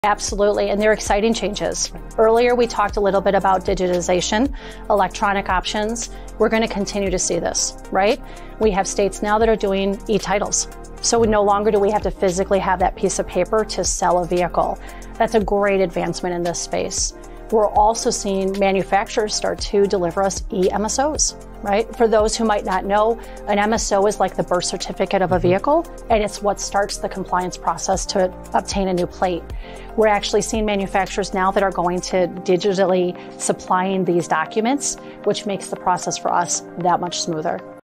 Revolutionizing Vehicle Documentation Going Paperless for Faster Transactions
In this short clip, our experts provide tips for tackling today's most pressing compliance issues.